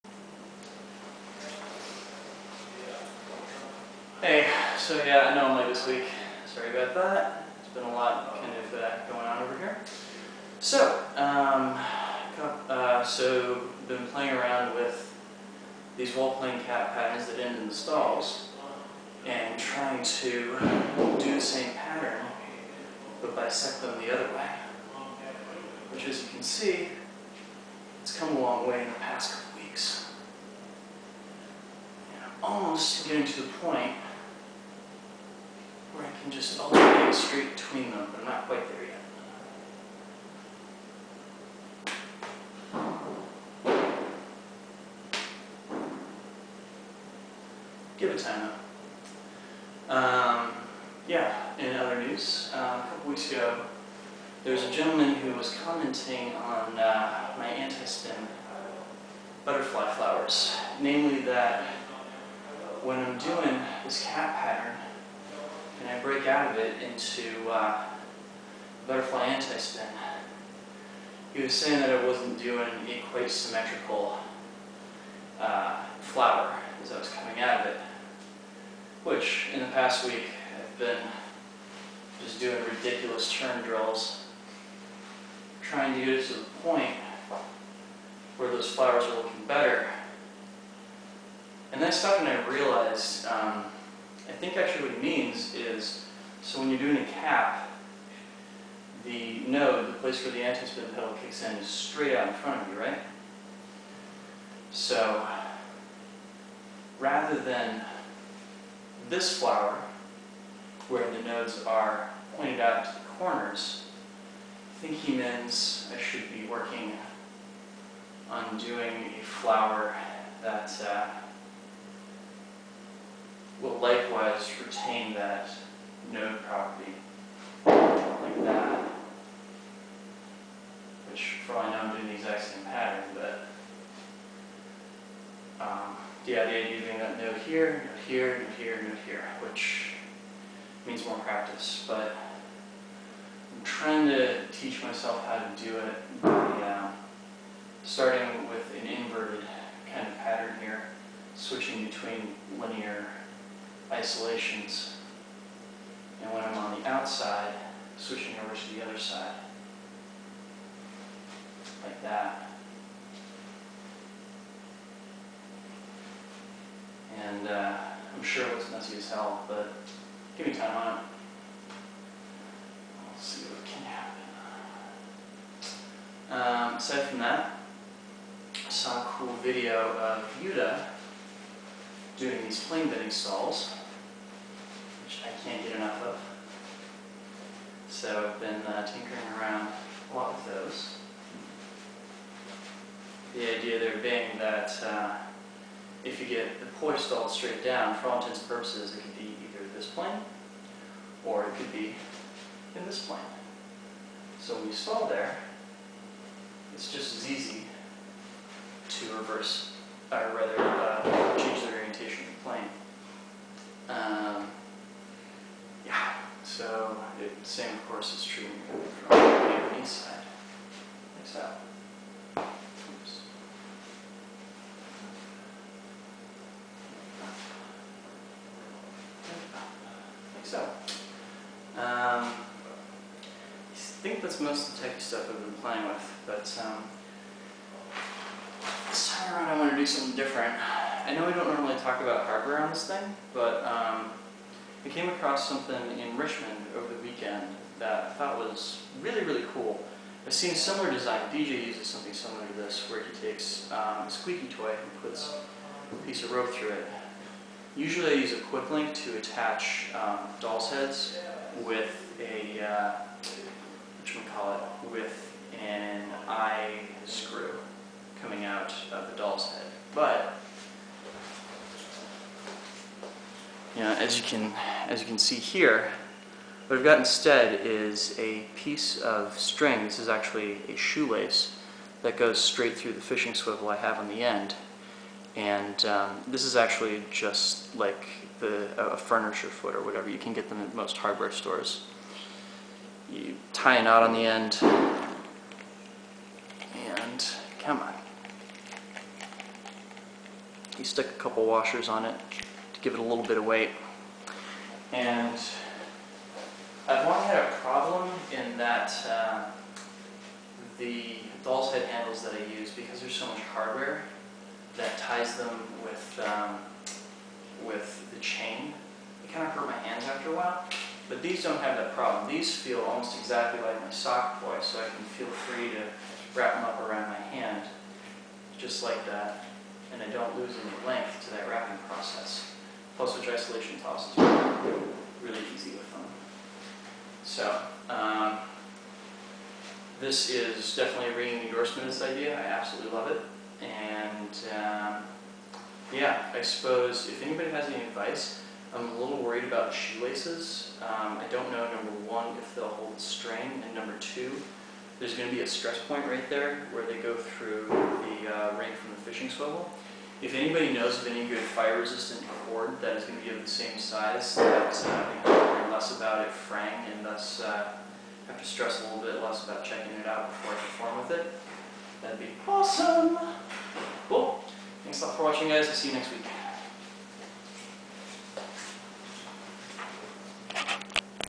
Drex's Tech Poi Blog #26: the CAP cross, butterfly anti-spin flowers, and a word on hardware
I've cleaned up the top and bottom CAPs from last week such to the point they're at least recognizable as this now. The next step will be to integrate them with the CAP-float pattern Insignia and I have been playing with to be able to do both the diamond hybrid pattern and bisect it like a cross as Zan is doing in the Arizona Transmission video. I got some feedback on my butterfly antispin flowers--especially in how they've been integrating into my CAP patterns and have come to realize I'm doing the wrong kind of flower. Back to the drawing board on that one. Finally, in Richmond this past weekend I came across a set of poi that had handles I thought were really nifty and innovative. I made a set for myself and I'm really happy with the results.